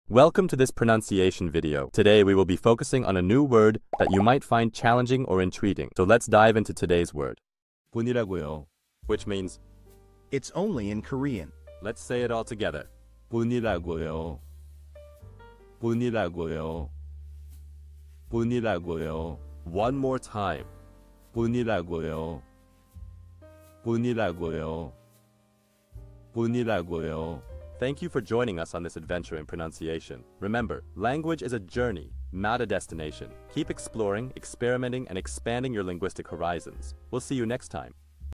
How to pronounce ppuniragoyo | 뿐이라고요 (It's only in Korean)
Master the Pronunciation of 'ppuniragoyo | 뿐이라고요' - which means : It's only in Korean 📝 with @PronunciationKorean 🎓🗣️ - [Your Guide to Korean]

Learn to pronounce 'ppuniragoyo' perfectly in Korean with our easy and engaging tutorial by @PronunciationKorean! 📖📽️ In this comprehensive video, we provide you with native speaker examples 🗣️👂 and phonetic breakdowns 🎵 to ensure you get the pronunciation just right.